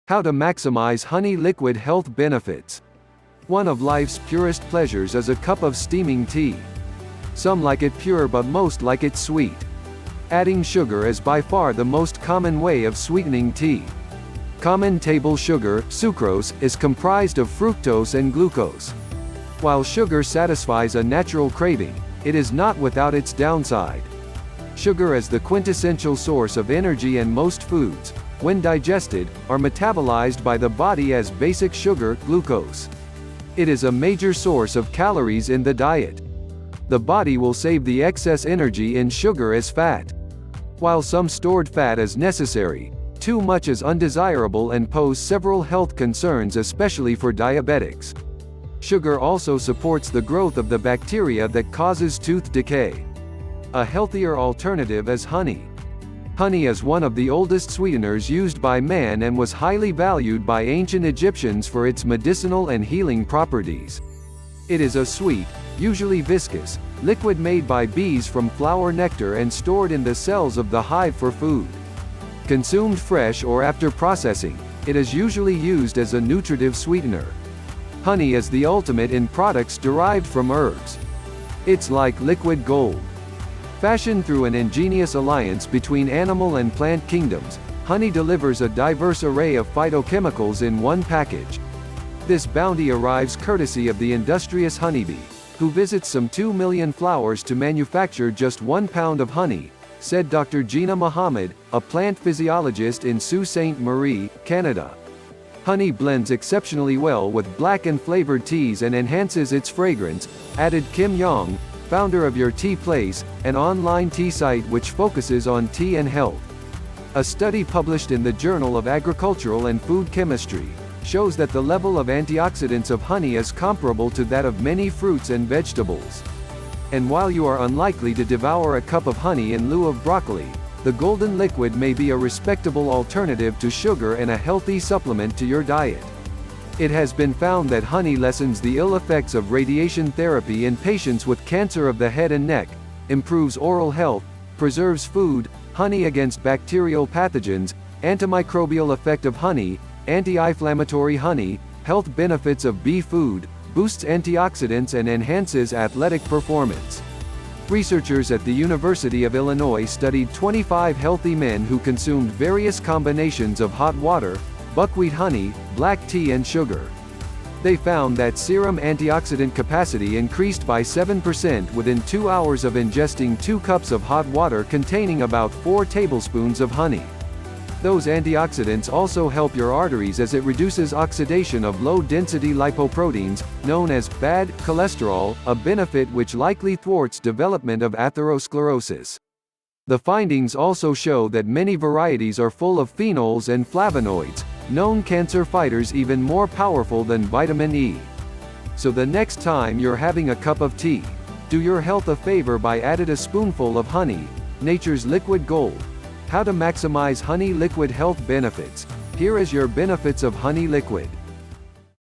HOW TO MAXIMIZE HONEY LIQUID FOR HEALTH BENEFITS
Maximize benefits of honey is the only product which contains and health, one of life’s honey purest pleasures is a cup of steaming tea , how to maximize honey liquid for health benefits.                                                                                       Some like it pure but most like it sweet. Adding sugar is by far the most common way of sweetening tea. Common table sugar (sucrose) is comprised of fructose and glucose. While sugar satisfies a natural craving, it is not without its downside.
00:00 intro
0:15 honey liquid health benefits
 supper black ice honey liquid health benefits.                                                                                                                                                                   Sugar is the quintessential source of energy and most foods, when digested, are metabolized by the body as basic sugar (glucose). It is a major source of calories in the diet. The body will save the excess energy in sugar as fat. While some stored fat is necessary, too much is undesirable and pose several health concerns especially for diabetics. Sugar also supports the growth of the bacteria that causes tooth decay.

A healthier alternative is honey. Honey is one of the oldest sweeteners used by man and was highly valued by ancient Egyptians for its medicinal and healing properties. It is a sweet, usually viscous, liquid made by bees from flower nectar and stored in the cells of the hive for food. Consumed fresh or after processing, it is usually used as a nutritive sweetener.

“Honey is the ultimate in products derived from herbs. It’s like liquid gold. Fashioned through an ingenious alliance between animal and plant kingdoms, honey delivers a diverse array of phytochemicals in one package. This bounty arrives courtesy of the industrious honeybee, who visits some 2 million flowers to manufacture just one pound of honey” said Dr.Gina Mohammed, a plant physiologist in Sault Ste Marie, Canada. “Honey blends exceptionally well with black and flavored teas and enhances its fragrance”, added Kim Yong, founder of Your Tea Place, an online tea site which focuses on tea and health.

A study published in the Journal of Agricultural and Food Chemistry, shows that the level of antioxidants of honey is comparable to that of many fruits and vegetables. And while you are unlikely to devour a cup of honey in lieu of broccoli, the golden liquid may be a respectable alternative to sugar and a healthy supplement to your diet. It has been found that honey lessens the ill effects of radiation therapy in patients with cancer of the head and neck, improves oral health, preserves food, honey against bacterial pathogens ,antimicrobial effect of honey, anti-inflammatory honey, health benefits of bee food, boosts antioxidants and enhances athletic performance.
Researchers at the University of Illinois studied 25 healthy men who consumed various combinations of hot water, buckwheat honey, black tea and sugar. They found that serum antioxidant capacity increased by 7 percent within two hours of ingesting 2 cups of hot water containing about 4 tablespoons of honey. Those antioxidants also help your arteries as it reduces oxidation of low-density lipoproteins (known as “bad” cholesterol), a benefit which likely thwarts development of atherosclerosis. The findings also show that many varieties are full of phenols and flavonoids – known cancer fighters even more powerful than vitamin E.

So the next time you’re having a cup of tea, do your health a favor by added a spoonful of honey, nature’s liquid gold. How to maximize honey liquid health benefits .Here is your benefits of honey liquid.